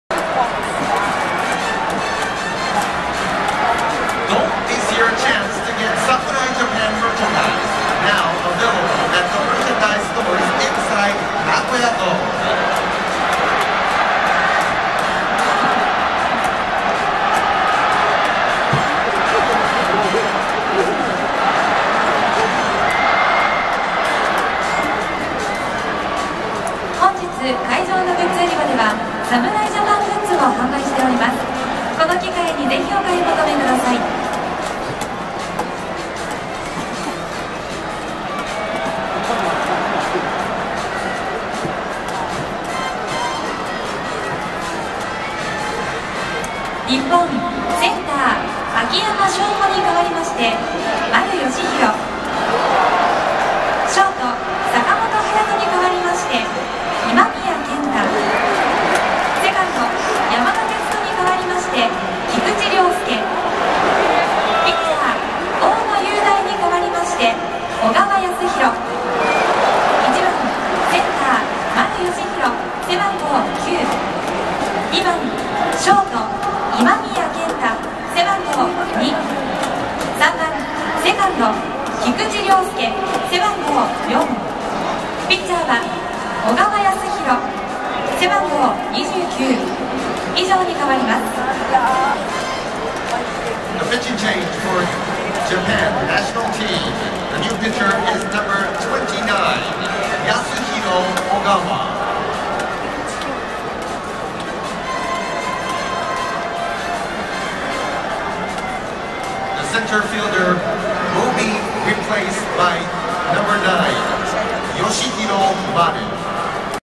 2016.3.5 侍ＪＡＰＡＮ ドアラ・つば九郎・たまべヱ・クマタン＆クマタンの彼女。
グッズを投げ入れたり、バズーカだったり。